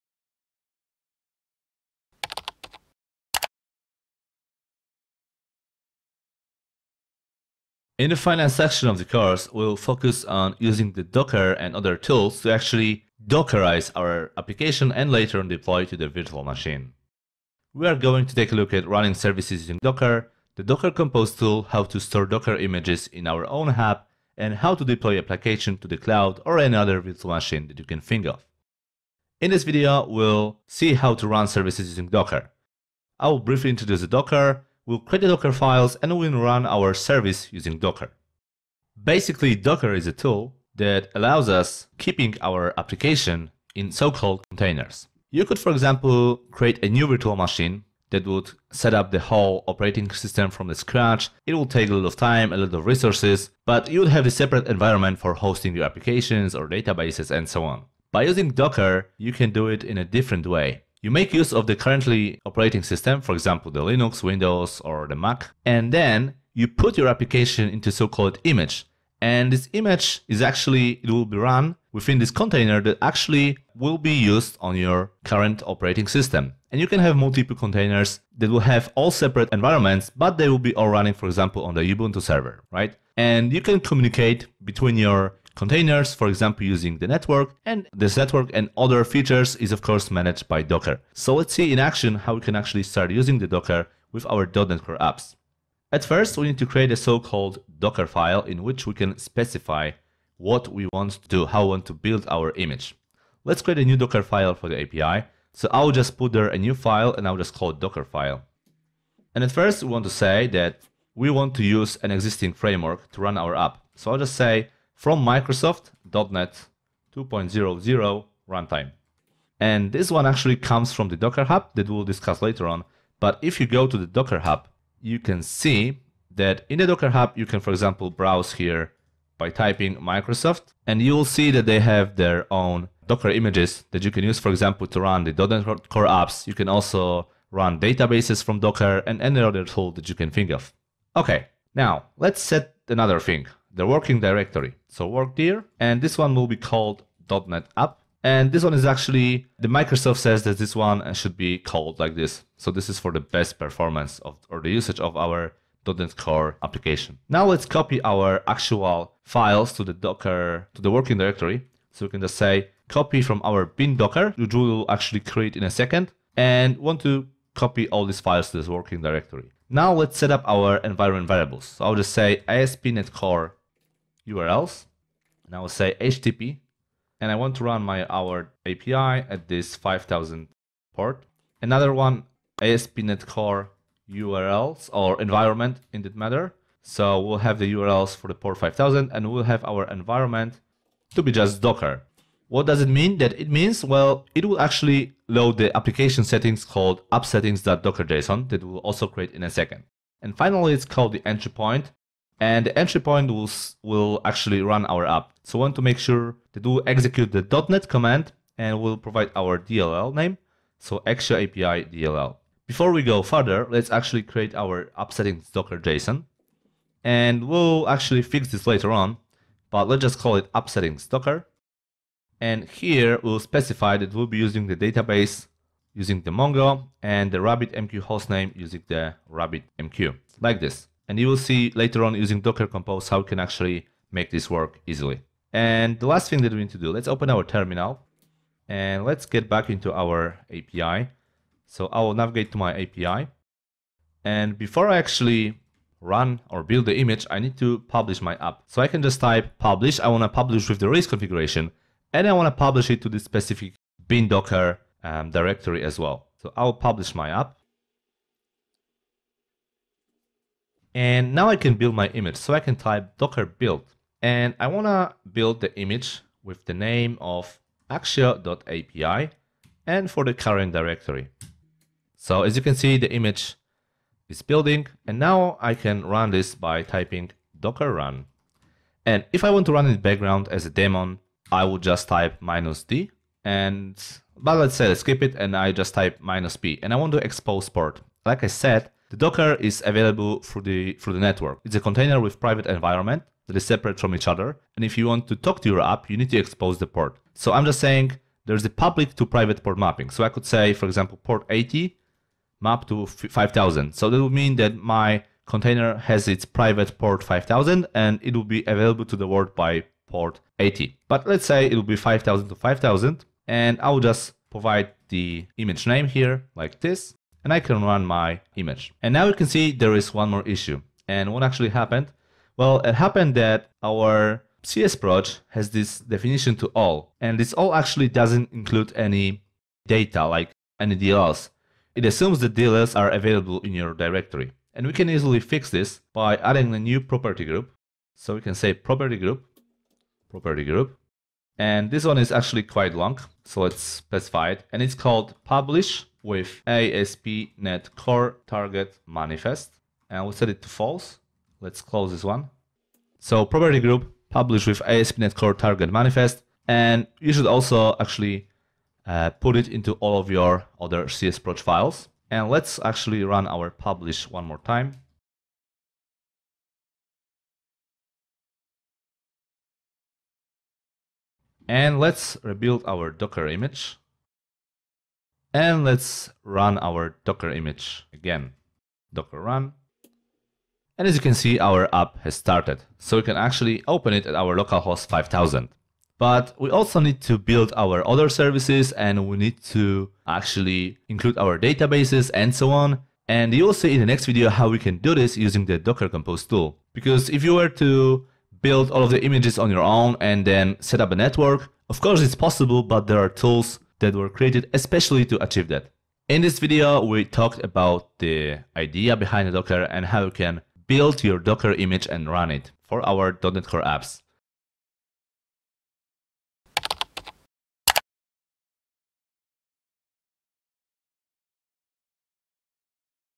.NET Core Microservices : Running Services Using Docker | packtpub.com
In this video, you will be introduced Docker.
• Introduce the Docker
• Create the Dockerfile
• Build and run the Docker image for API